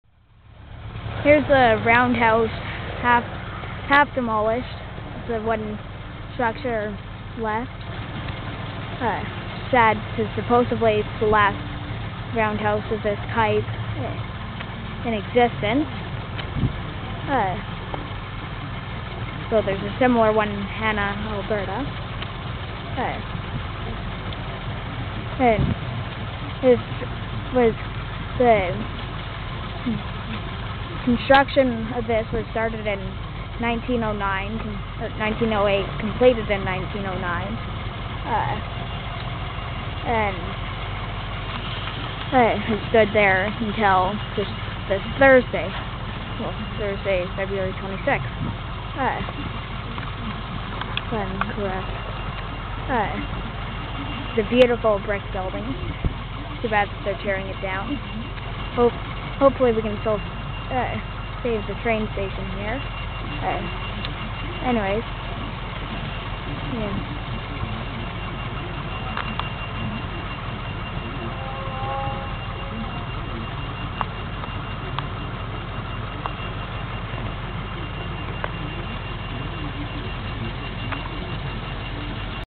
Biggar's Roundhouse's Remains
This is all that is left of the Last GTPR Roundhouse in the World, which was located in Biggar, Saskatchewan. Demolition started on Thursday, Feb. 26, 2015, and continued through today. The roundhouse was built from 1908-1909 and was expanded in 1923. It was built because Biggar was going to be a major railway terminal for switching crews between Edmonton and Winnipeg.
The roundhouse was abandoned in 1950 and slated for demolition in 1971. Luckily, the demolition was postponed while a lease was being signed. In 1975, The 2nd largest turkey farm in Saskatchewan opened, and was in use until 2005. From then until 2009, it was used for grain storage, and then was once again slated for demolition. However, this was once again postponed (then canceled) because mayor Ray Sadler and Rey Chamberland collected over 3,000 signatures (and a few thousand pigeons, rats, etc. that had moved in) to save it. In 2014, the residents of Biggar were notified that if not enough people supported saving the nearby train station, it was to be demolished (it hasn't been yet). Then in late January, the roundhouse was under a demolition permit and was torn down a month later.